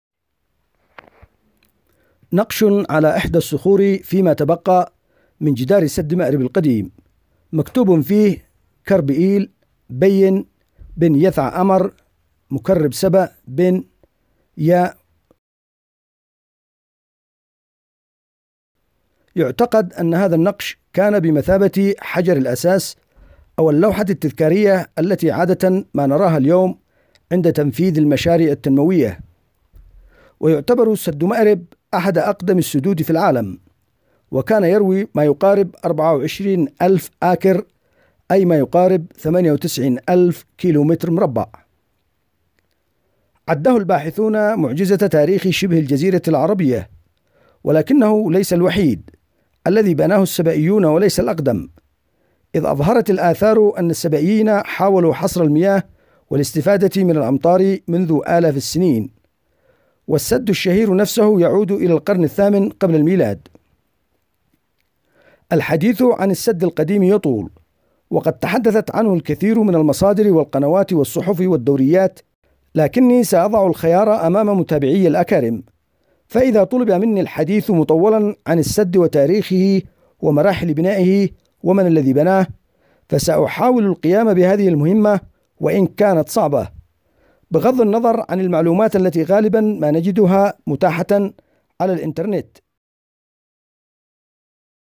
حجر الأساس لبناء سد مأرب🔥
اللوحة التذكارية اليمنيه :

نقش على إحدى الصخور في ما تبقى من جدار سد مأرب ..
مكتوب فيه :

(كرب إل / بيِّن / بن / يثع أمر / مكرب / سبأ / بن / ي .)

أعتقد أن هذا النقش كان بمثابة حجر الأساس أو اللوحه التذكارية
التي نراها عند تنفيذ المشاريع التنموية في الوقت الراهن ..

سد مارب القديم

سد مأرب هو سد مائي يقع في محافظة مأرب اليمنية، ويعود تاريخ السد القديم إلى بدايات الألفية الأولى ق.م، ويعتبر سد مأرب أحد أقدم السدود في العالم وكان يروي ما يقارب 24,000 آكر (قرابة 98,000 كم مربع) وعده الباحثون معجزة تاريخ شبه الجزيرة العربية، ولكنه ليس الوحيد الذي بناه السبئيون وليس الأقدم إذ أظهرت الآثار أن السبئيين حاولوا حصر المياه والاستفادة من الأمطار منذ الألفية الرابعة ق.م، إلا أن السد الشهير نفسه يعود إلى القرن الثامن ق.م

بني السد من حجارة أقتطعت من صخور الجبال، حيث نحتت بدقة، ووضعت فوق بعضها البعض واستخدم الجبس لربط الحجارة المنحوتة ببعضها البعض، واستخدام قضبان إسطوانية من النحاس والرصاص يبلغ طول الواحدة منها ستة عشر متراً، وقطرها حوالي أربع سنتمرات توضع في ثقوب الحجارة فتصبح كالمسمار فيتم دمجها بصخرة مطابقة لها وذلك ليتمكن من الثبات أمام خطر الزلازل والسيول العنيفة حسب التنقيبات الأثرية، فإن السد تعرض لأربع انهيارات على الأقل  آخرها كان في العام 575 ولم تبذل جهود لترميمه من ذلك الحين بعد لغياب حكومة مركزية واضطراب الأمن في البلاد وتدخل القوى الأجنبية (الفرس) واستقلال زعماء القبائل بإقطاعياتهم

بناء السد القديم

هناك اختلاف بين الباحثين حول مرحلة بناء «سد مأرب العظيم»، فقد مر بعدة أطوار وأبحاث أثرية حديثة قام بها المعهد الألماني للآثار تشير إلى نظم زراعية قرب مأرب تعود إلى الألفية الرابعة ق.م، إلا أن السد الشهير نفسه يعود إلى القرن الثامن ق.م وبقي صامدا يؤدي عمله حتى عام 575م،  بعض الباحثين أعاده إلى القرن العاشر ق.م.تعرض السد لعدة تصدعات ولكن نهايته كانت في العام 575 بعد الميلاد لغياب حكومة مركزية واضطراب الأمن في البلاد وتدخل القوى الأجنبية (الفرس) واستقلال زعماء القبائل بإقطاعياتهم.
في القرن الثامن، وردت نصوص لمكاربة تشير إلى تعلية وإصلاحات على السد فبلغ طوله 577 متراً وعرضه 915 متر وهو ضعف سد هوفر الأمريكي،  وبني في الجهة التي تسيل منها السيول فتمكن السد من حصر الماء وزود بثقوب أو أبواب لتسمح بقدر أكبر من التحكم بجهة المياه عقب استقرارها في الحوض. وتم اقتطاع حجارة السد من صخور الجبال ونحتت بدقة ووضعت فوق بعضها البعض واستخدم الجبس لربط الحجارة المنحوتة ببعضها البعض واستخدام قضبان إسطوانية من النحاس والرصاص يبلغ طول الواحدة منها ستة عشر مترا وقطرها حوالي أربع سنتمرات توضع في ثقوب الحجارة فتصبح كالمسمار فيتم دمجها بصخرة مطابقة لها وذلك ليتمكن من الثبات أمام خطر الزلازل والسيول العنيفة  كان يروي ما يقارب 24,000 آكر (قرابة 98 كم مربع)  وهو ماعنى أن سكان اليمن حققوا الاكتفاء الذاتي من ناحية احتياجهم للماء والغذاء، فالماء الناتج عن السد القديم كان يكفيهم للزراعة وإطعام مواشيهم.
ويعتبر سد مأرب من أرقى السدود من الناحية الهندسية. وقد قام المهندسون بمعاينة طبيعة الأرض قبل إنشاء السد، ثم بنوا عليها المخطط الهندسي الذي هو عبارة عن حائط حجري ضخم أقيم في مربط الدم عند مخرج السيل من الوادي، وبني على زاوية منفرجة، ممتدًا من الجنوب إلى الشمال مسافة ستمائة وخمسين مترًا، وله فتحات وأبواب تُفتح وتُغلق حسب الحاجة لمرور الماء منها المسايل المتصلة بها لإرواء الحقول والبساتين والمزارع.

تهدم السدعدل

تدمير سد مأرب حدثا تاريخيا، وذكر في القرآن الكريم:

لَقَدْ كَانَ لِسَبَإٍ فِي مَسْكَنِهِمْ آيَةٌ ۖ جَنَّتَانِ عَن يَمِينٍ وَشِمَالٍ ۖ كُلُوا مِن رِّزْقِ رَبِّكُمْ وَاشْكُرُوا لَهُ ۚ بَلْدَةٌ طَيِّبَةٌ وَرَبٌّ غَفُورٌ، فَأَعْرَضُوا فَأَرْسَلْنَا عَلَيْهِمْ سَيْلَ الْعَرِمِ وَبَدَّلْنَاهُم بِجَنَّتَيْهِمْ جَنَّتَيْنِ ذَوَاتَيْ أُكُلٍ خَمْطٍ وَأَثْلٍ وَشَيْءٍ مِّن سِدْرٍ قَلِيلٍ

ترتب على ذلك فشل نظام الري، ما أدى لهجرة ما يصل إلى 50,000 شخصاُ من اليمن إلى مناطق أخرى من شبه الجزيرة العربية

في 2 أكتوبر 1984م تم وضع حجر الأساس للسد، وفي أواخر 1986م أعلن رئيس دولة الإمارات الراحل الشيخ زايد بن سلطان آل نهيان تبرعه بإعادة بناء وتأهيل وتوسيع سد مأرب، وفي عام 2003م بدأ العمل في المرحلة الثانية من مشروع قنوات إعادة تأهيل سد مأرب بطول 69 كيلو متراً بتكلفة بلغت 23 مليون و910 آلاف دولار أمريكي بتمويل من صندوق أبو ظبي للتنمية.

تبلغ مساحة بحيرة السد 30 كيلومتر مربع، ويسع 400 مليون متر مكعب من الماء، فيما تعمل بوابة التصريف الخاصة به بطاقة قدرها 35 مترا مكعبا في الثانية وذلك لري حوالي 16 ألف هكتار من الأراضي الزراعية، ويبلغ عمق الجسم الخرساني للسد 60 مترا ومساحته 24 ألف متر مربع وطوله 763 مترا وعرضه عند مستوى سطح الوادي 337 مترا، وعند مخرج المياه من بوابة التصريف 195 مترا كما بلغ إجمالي حجم الردميات في جسم السد 3 ملايين متر مكعب، وتم إزالة 200 ألف متر مكعب من الصخور من جانبي الموقع وتغطية جسم السد بصخور بلغ حجمها 100 ألف متر مكعب.[12]
في أغسطس 2020 شهد السد فيضان المياه من الأماكن المخصصة لذلك وهي المرة الأولى التي تتجاوز كمية المياه السعة القصوى للسد منذ إعادة بنائه قبل 34 عاماً، حيث تجاوز منسوب المياه حاجز الـ 550 مليون متر مكعب.[13]

بقايا آثار سد مأرب العظيم

الموقع محافظة مأرب، اليمن
المنطقة شبه الجزيرة العربية
إحداثيات 15°23′46″N 45°14′37″E
النوع سد مائي
الطول577 متراً العرض915 مترالباني
السبئيون المادة حجارة من صخور الجبال والجبسبُني
القرن الثامن ق.م
الحضارات مملكة سبأ
الملكية الجمهورية اليمنية
المتصرف وزارة الثقافة
الاتاحة للجمهور متاح

آثار قدماء#